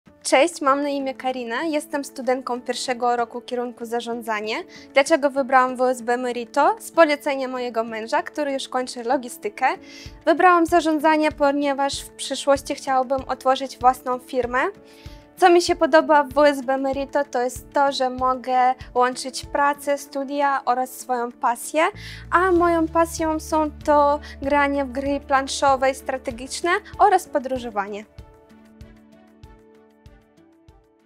Opinia studentki na temat studiów na kierunku zarządzanie na Uniwersytecie WSB Meirto we Wrocławiu
Karinie studia na Uniwersytecie WSB Merito we Wrocławiu polecił jej mąż, nasz student kierunku logistyka. Co najbardziej ceni sobie w studiowaniu na naszej uczelni? Posłuchaj jej wypowiedzi!